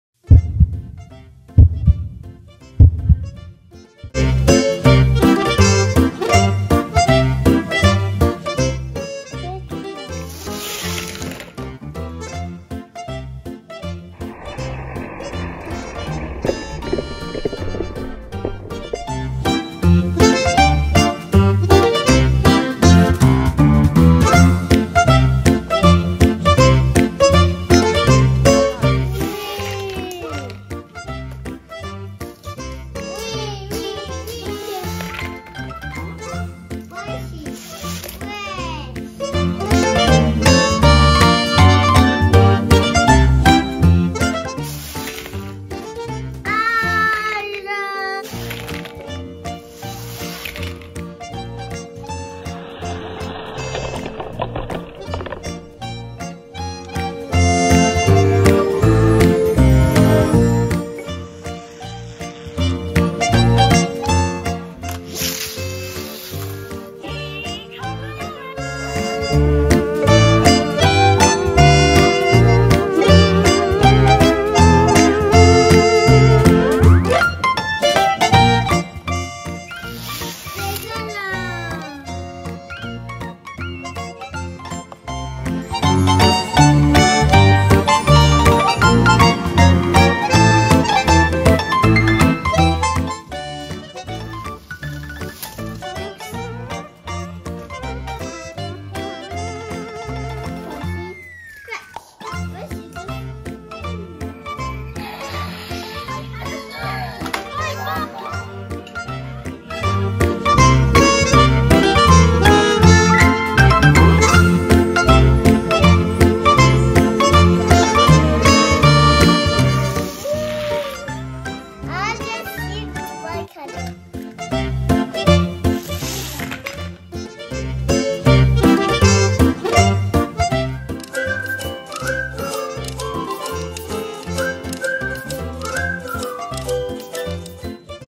Thomas & Friends Crash!DIESEL TUNNEL BLAST!
In this video Vlad played with Diesel tunnel blast set! We had a great time playing and filming!